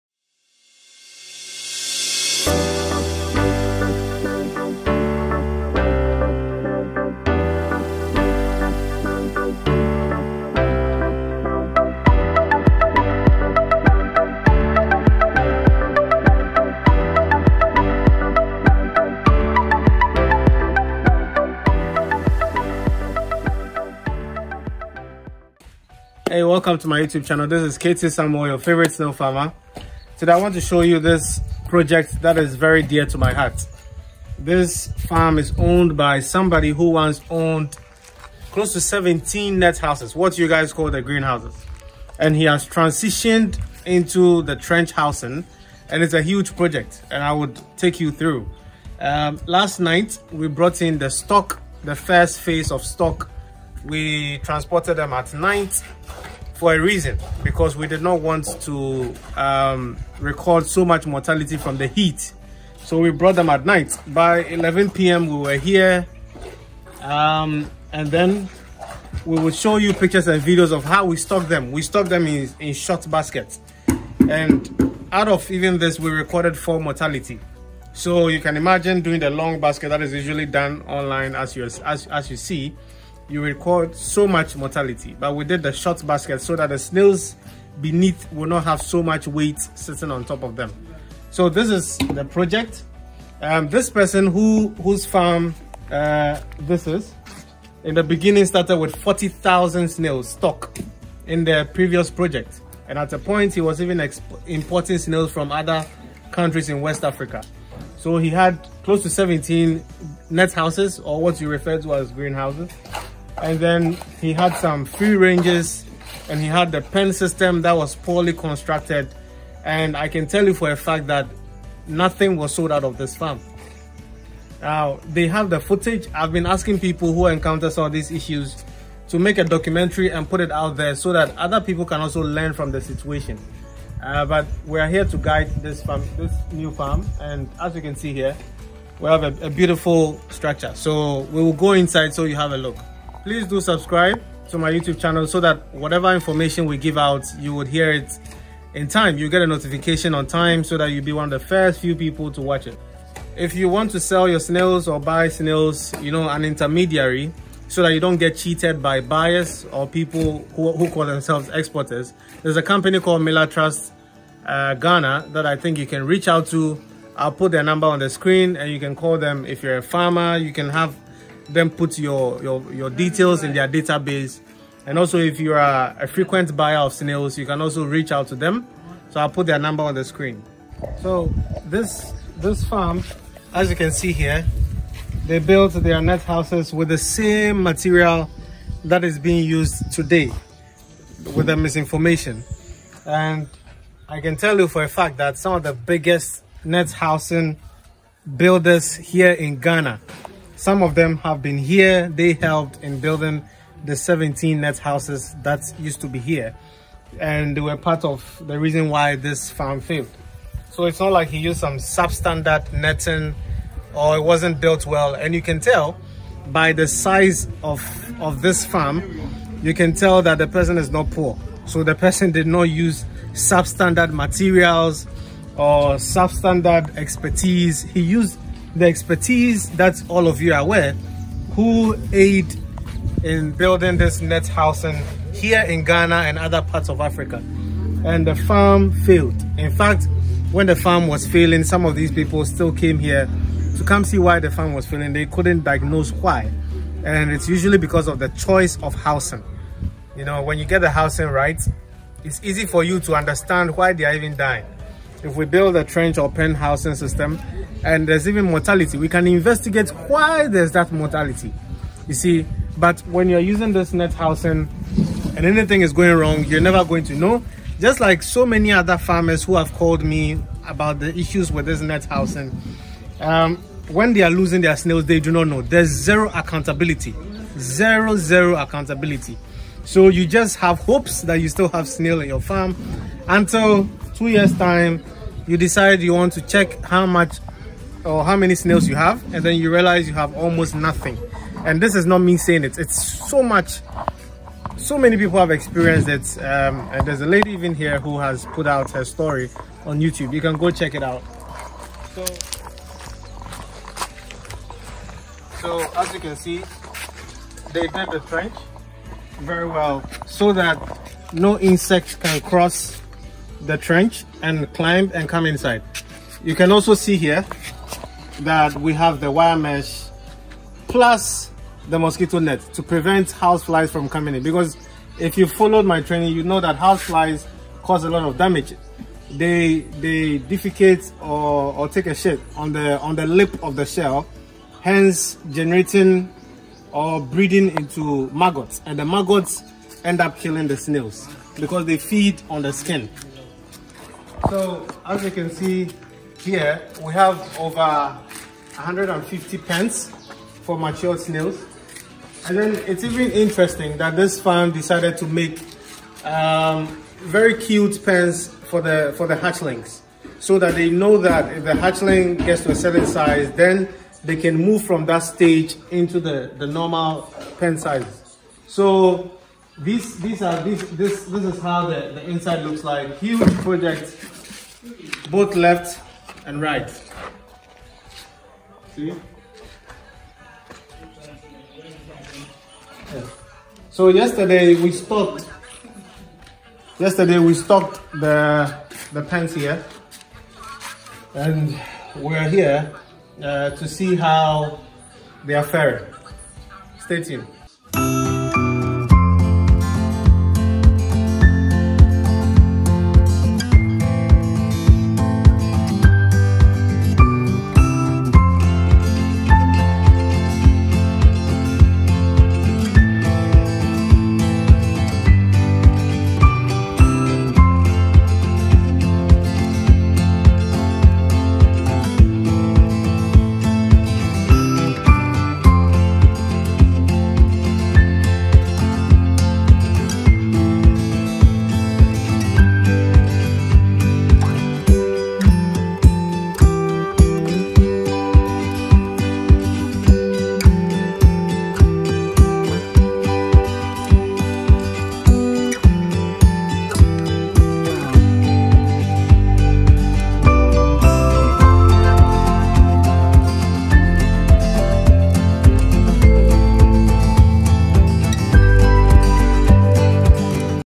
He built 17 Snail "greenhouses" and lost all snails so switched housing module.
In this video we shared the story of a snail farmer who invested close to $300,000 into various snail housings especially the snail greenhouse and lost all snails. Through my videos, he got encouraged to reignite the farm but this time with a housing that aids with accountability.